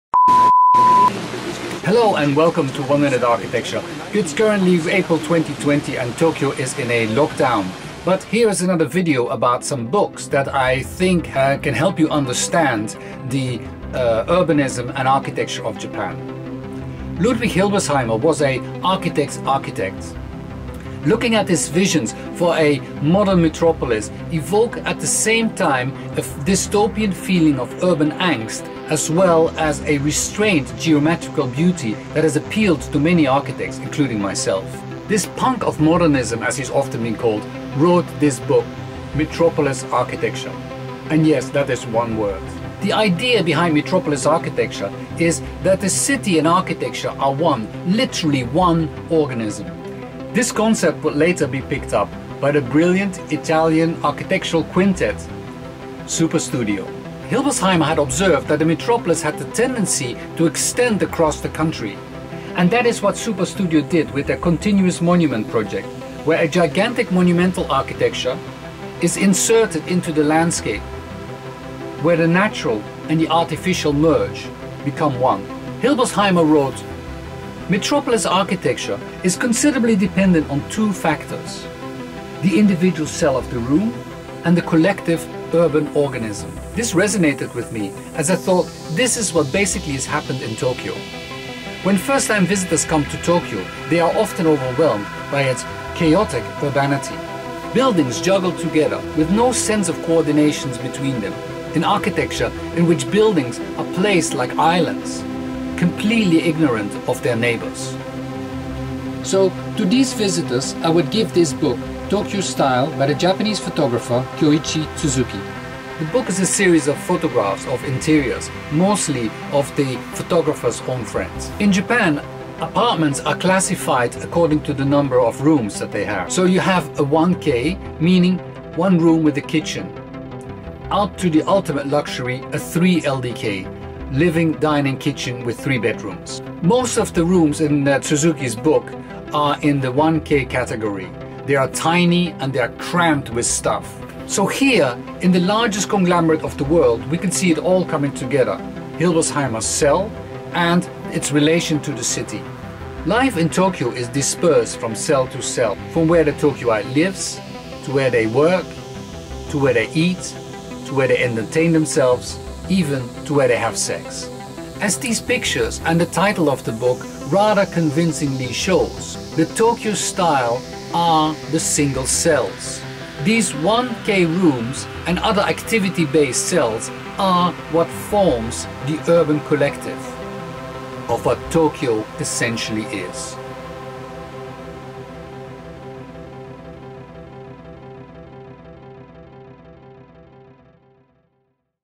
Tokyo Style - Explaining the urbanity and architecture of Japan (One Minute Architecture)
A review of Tokyo's architecture and its relation to its urbanity using Kyoichi Tsuzuki's Tokyo Style and Ludwig Hilbersheimer's Metropolisarchitecture.

I've been obsessed with Ludwig Hilbersheimer's quote that "Metropolisarchitecture is considered dependent on solving two factors: the individual cell of the room and the collective urban organism." This is what one can see in Tokyo. In Tsuzuki's book of photographs of the small interiors of his friends we can see that these are the individual cells, rooms which float within the space of the metropolis. A metropolis which itself is made of millions of cells, not just that of the cells of the individual rooms but of the cells of the activities that are spread throughout the city. Apart from activities such as work or eating out, other activities that one traditionally links to the home such as entertaining friends but also sex are taking place in cells outside of the home. Thus I have to agree that the single cell of the room and of the activity based cells are what defines the metropolitan urbanity of Tokyo.

Other books used: Peter Land and William Menking: Superstudio. I'm sad to hear that Menking passed away recently.